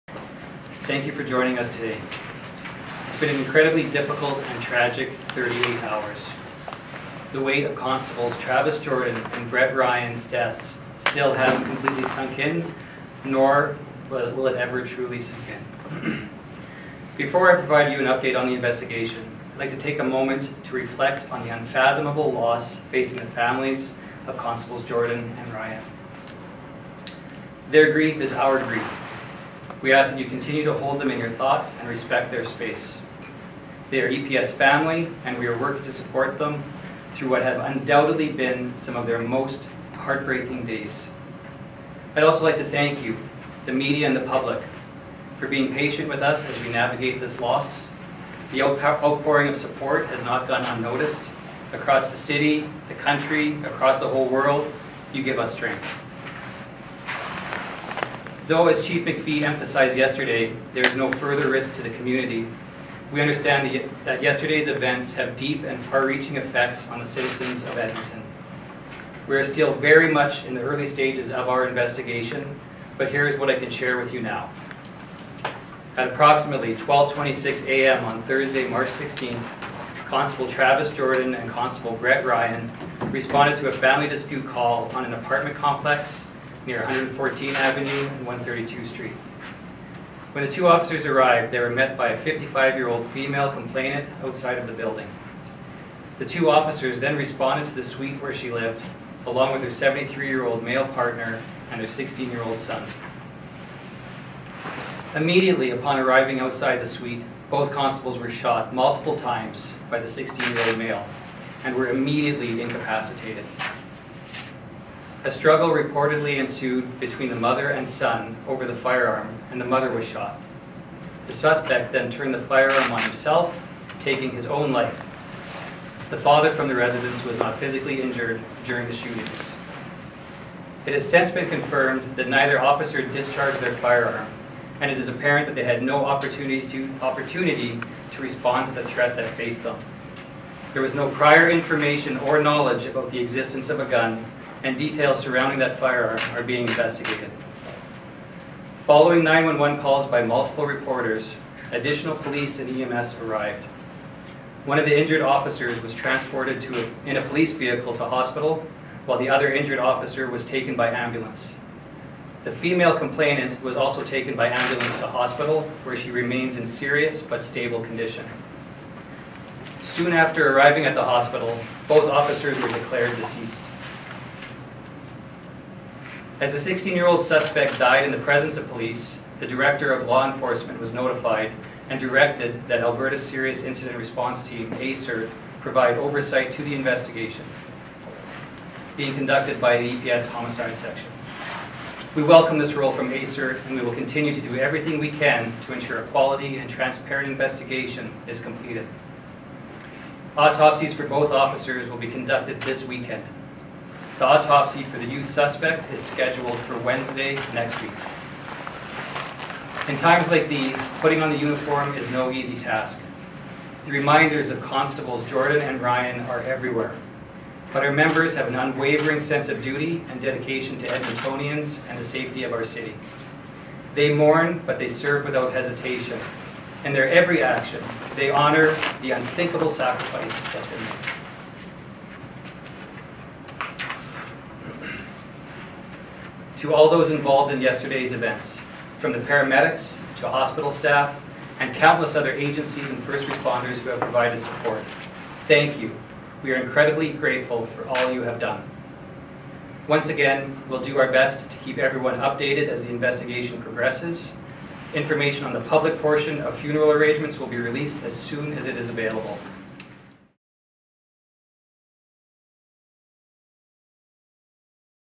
Update: Slain EPS officers didn't know teen shooter had a gun
A 16-year-old boy who killed himself after fatally shooting two Edmonton Police Service constables had been involved in a previous mental health call but had no criminal record, police officials said Friday.